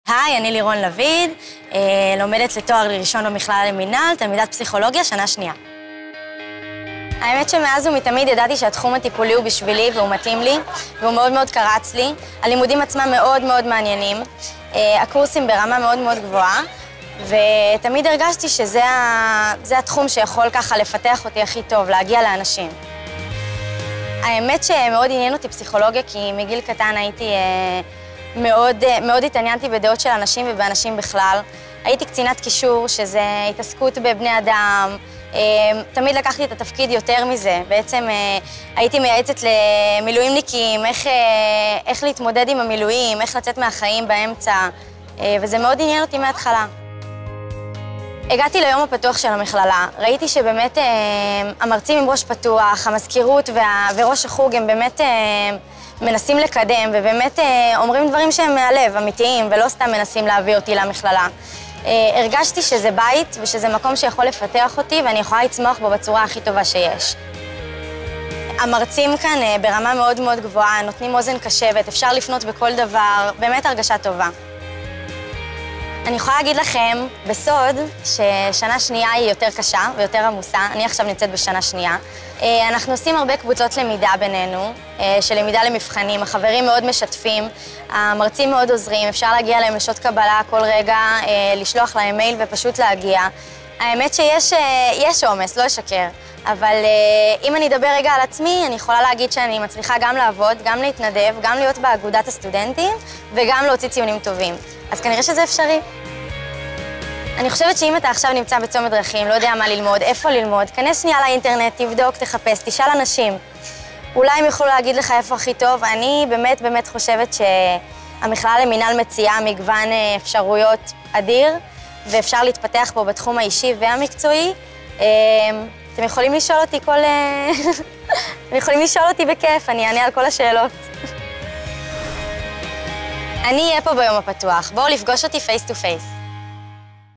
בואו להכיר את לירון ,תלמידת תואר ראשון בפסיכולוגיה
לירון ,תלמידת שנה ב' , תואר ראשון בפסיכולוגיה מהמסלול האקדמי המכללה למינהל, מביאה לכם טיפ אישי:" אם אתה עכשיו נמצא בצומת דרכים לא יודע מה ללמוד מה ללמוד ,איפה ללמוד- תבדוק, תחפש תשאל אנשים; אולי הם יוכלו להגיד לך איפה הכי טוב".